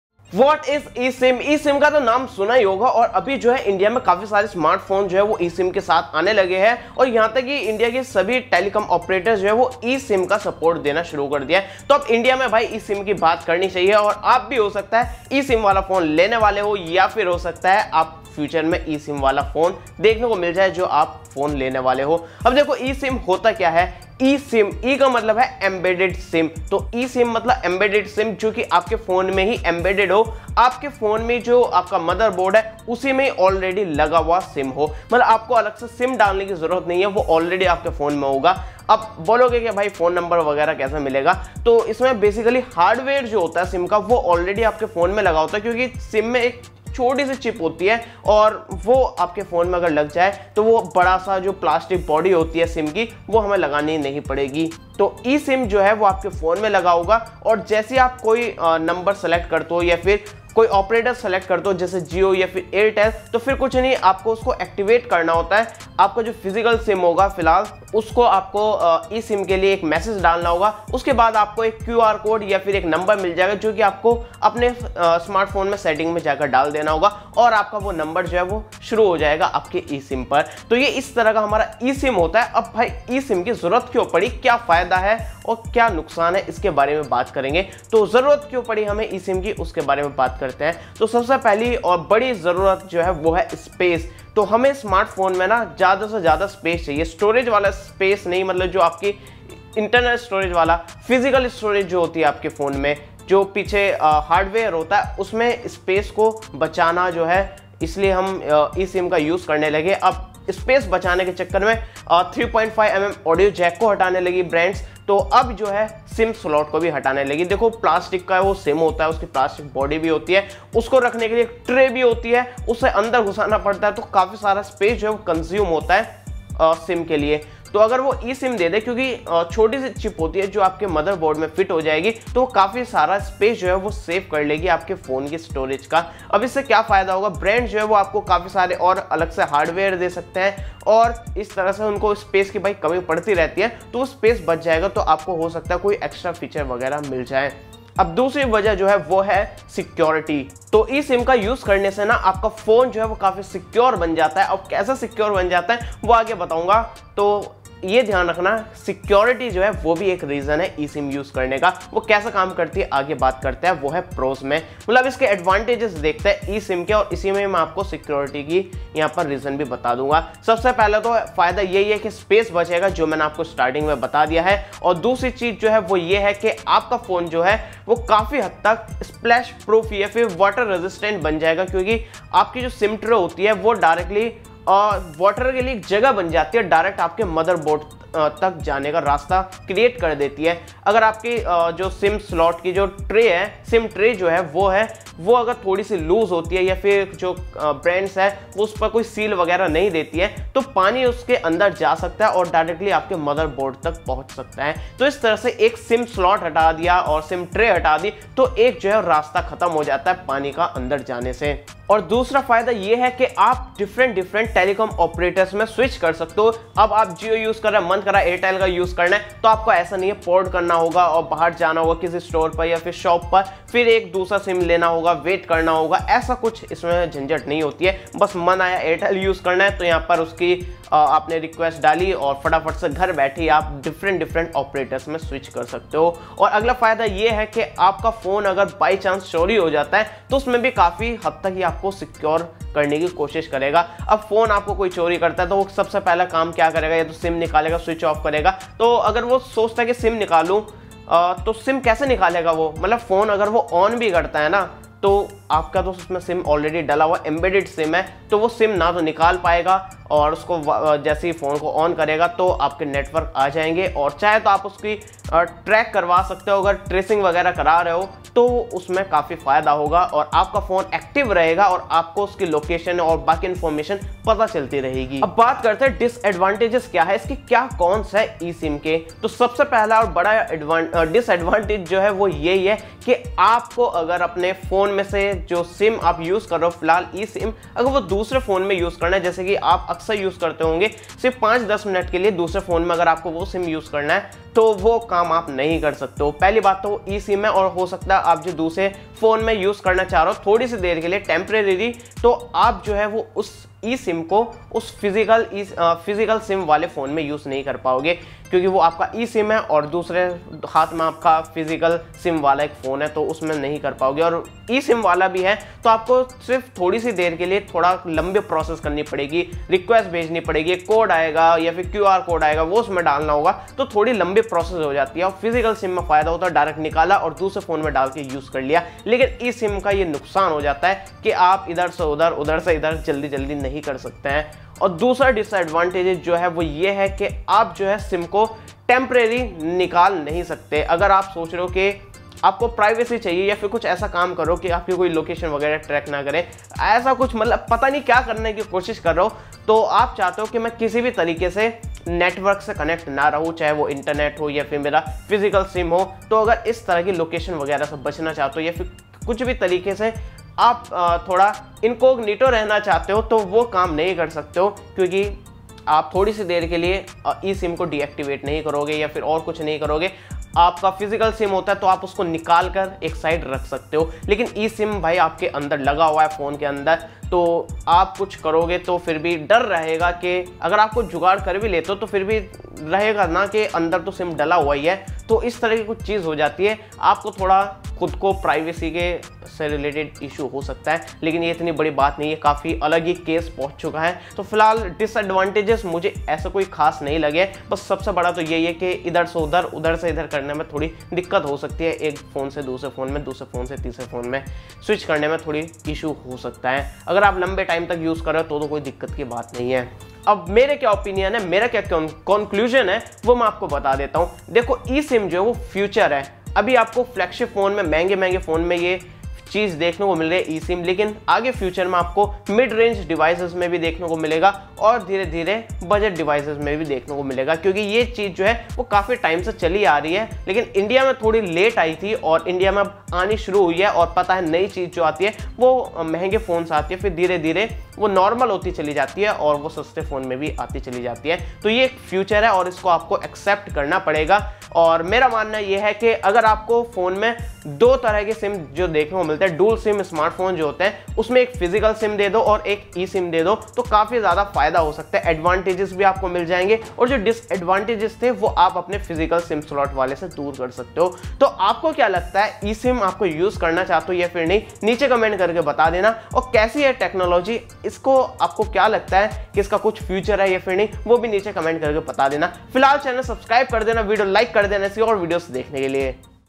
What is eSIM and How it Works | Explained in Hindi
📝 TOPICS COVERED:
1- What is eSIM in Hindi
2- How eSIM Works Hindi
3- eSIM Kya hain
4- eSIM Explained in Hindi

Chapters:
00:00 Intro
00:23 What is eSIM
01:22 Why eSIM?
02:45 Advantages of eSIM
04:34 Disadvantages of eSIM
06:50 My Opinion